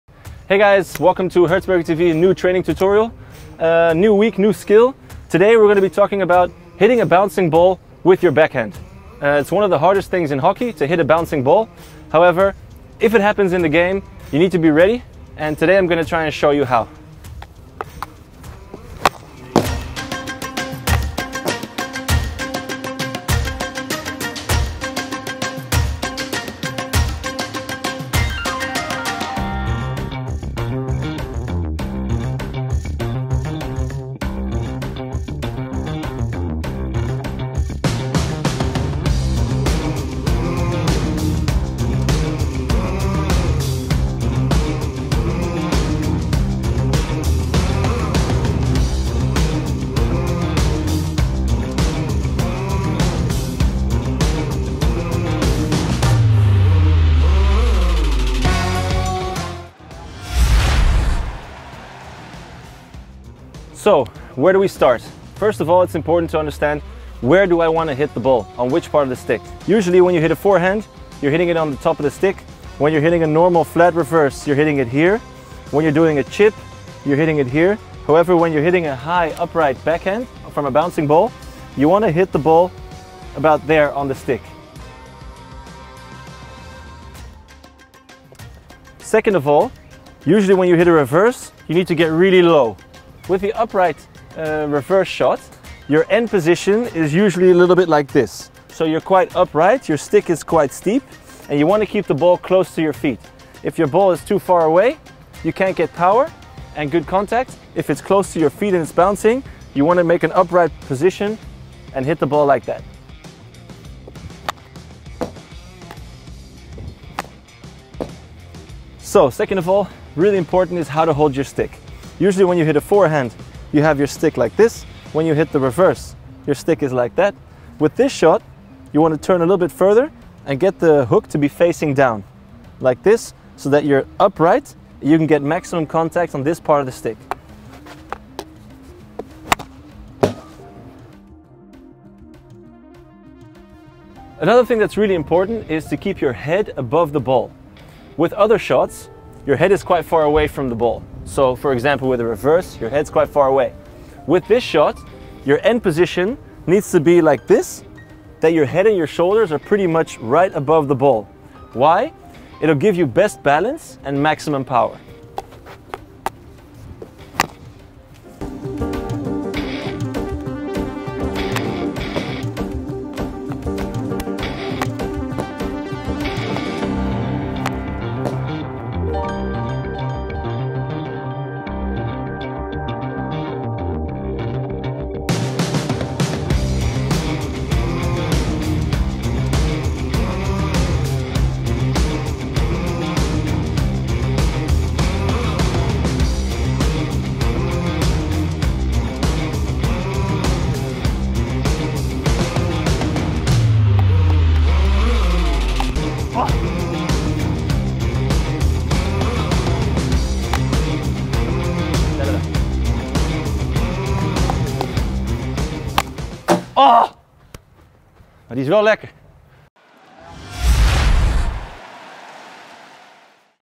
High Reverse/Backhand | Hertzberger TV | Field Hockey Training Tutorial
This weeks training tutorial is one of the hardest skills in hockey. The high backhand or reverse shot is a difficult one to use in the game but when you get the chance you need to be ready.